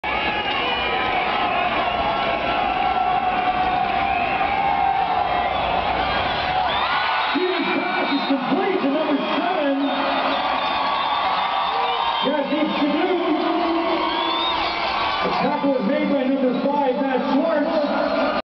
Sidhu great 24 yard reception
Midget Championship 2008, Mustangs vs. Nomads.